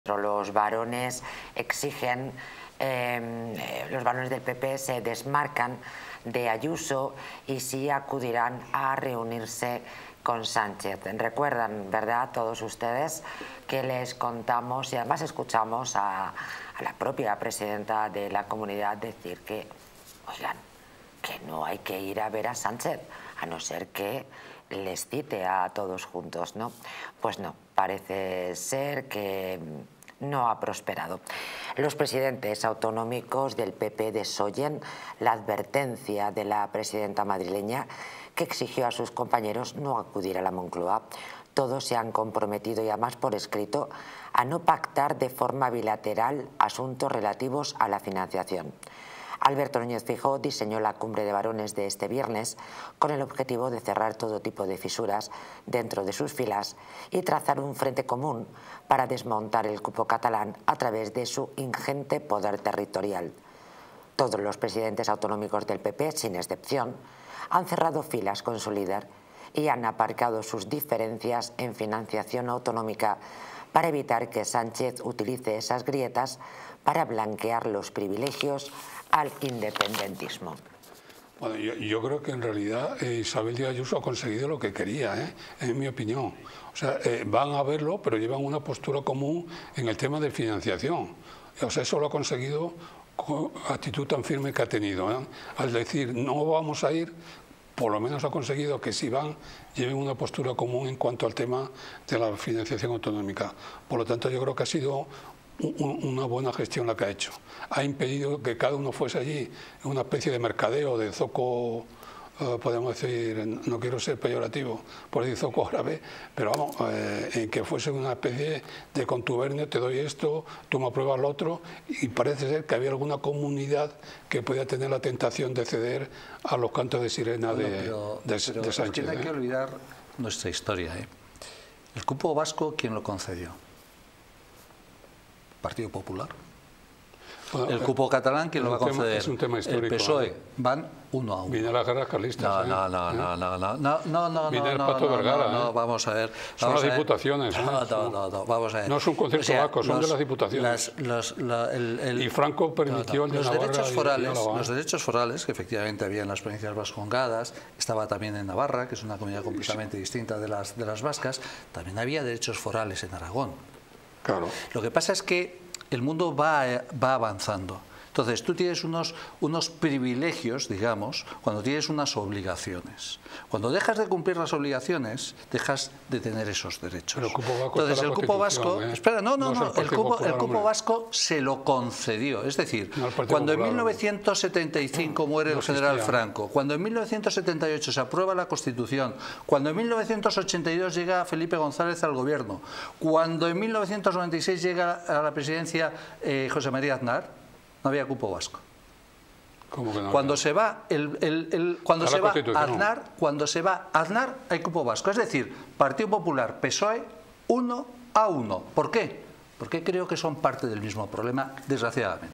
🔴Los barones del PP se desmarcan de Ayuso y sí acudirán a reunirse con Sánchez🔴
Los barones del PP se desmarcan de Ayuso y sí acudirán a reunirse con Sánchez en este video de DistritoTV. ¡No te lo pierdas! 🔴distritotv
🔴Conviértete en miembro de este canal para disfrutar de ventajas: